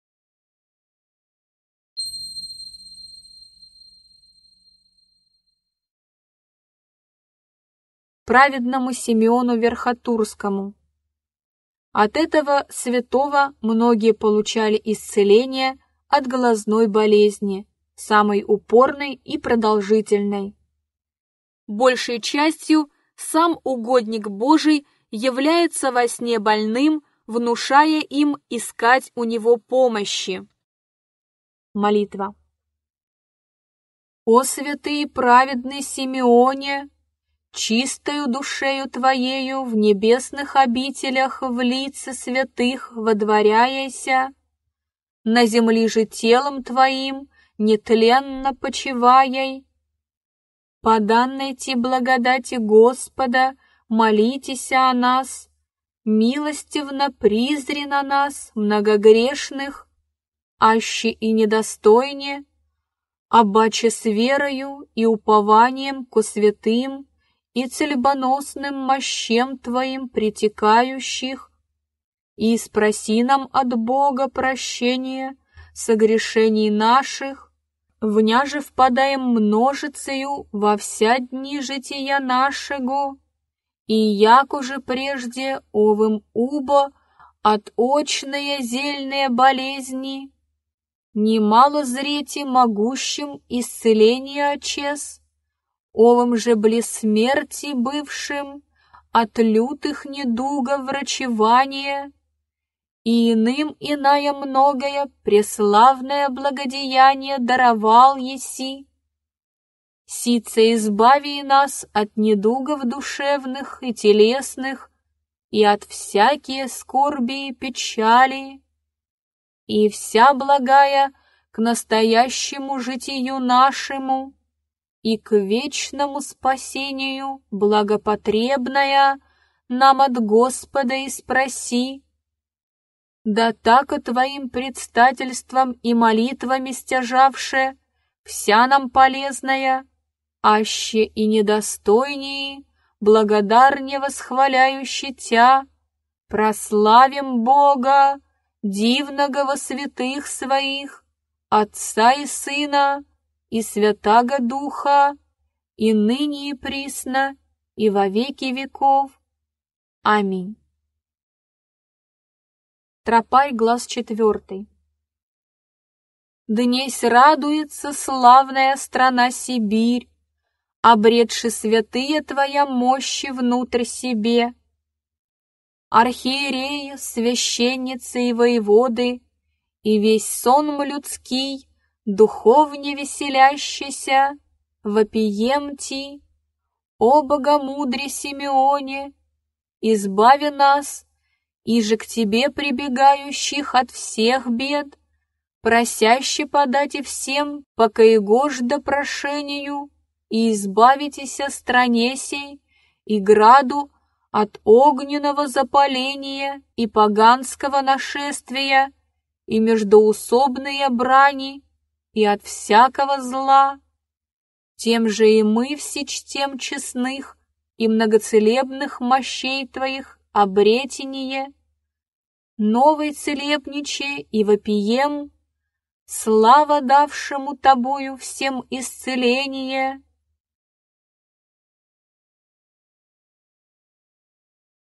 11.Праведному Симеону Верхотурскому. Молитвы при глазных болезнях.
От этого святого многие получали исцеление от глазной болезни, самой упорной и продолжительной. Большей частью сам угодник Божий является во сне больным, внушая им искать у него помощи.
Симеон Верхотурский (Меркушинский) стяжал свою святость в миру молитвами, отшельничеством, странничеством, трудами и помощью людям. Ныне по молитвам святого Симеона Верхотурского Господь являет благодатную помощь, утешение, укрепление, вразумление, врачевание душ и телес и избавление от лукавых и нечистых духов. Бедствующие путники по молитвам святого получают избавление от смерти. Особенно часто обращаются с молитвами к Верхотурскому чудотворцу при глазных болезнях и всевозможных параличах (это одно из первых чудес от его мощей).